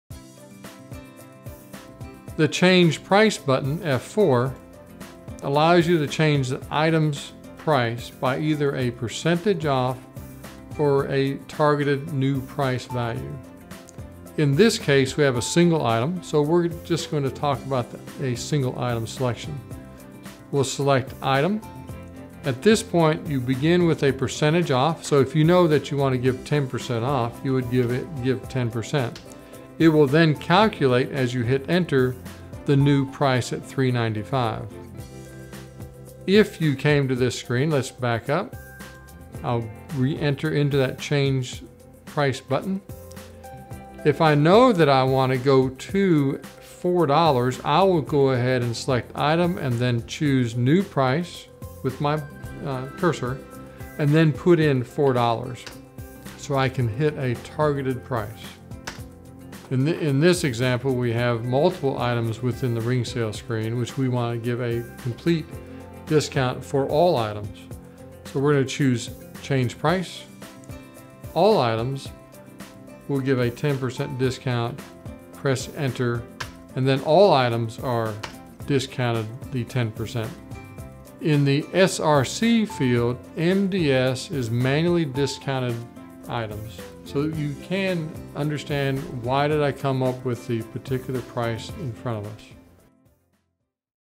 01-020-120 POS: Change Price
Change Price F4 allows the user to change the price of an item, if they have the security permission to complete this task.

Item - The Item button allows the user to change the price of a single item by a percentage or to a targeted new price.

All Items - The All Items allows the user to change the price of all items by the indicated percentage.

This is a manual method of discounting prices.  Multiple discount processes are available to address this topic.